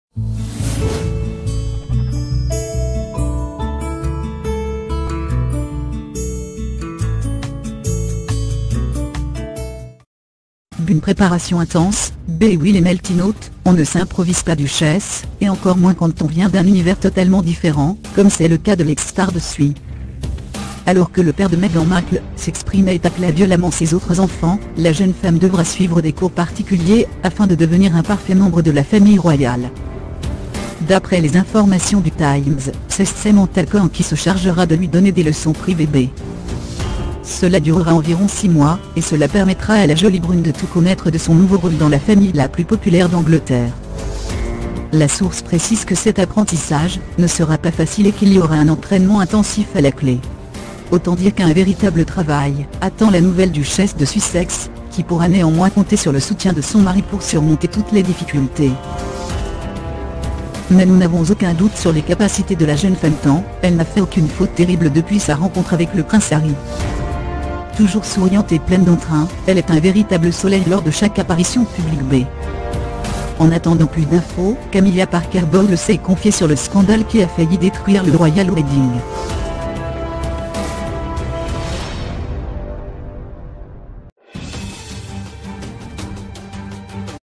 Meghan Markle s'apprête à passer six mois à apprendre codes royaux, tous les détails dévoilés !
bUne préparation intense ! b Eh oui les meltynautes, on ne s'improvise pas Duchesse et encore moins quand on vient d'un univers totalement différent comme c'est le cas de l'ex star de Suits.
 Alors que le père de Meghan Markle s'exprimait et taclait violemment ses autres enfants, la jeune femme devra suivre des cours particuliers afin de devenir un parfait membre de la famille royale.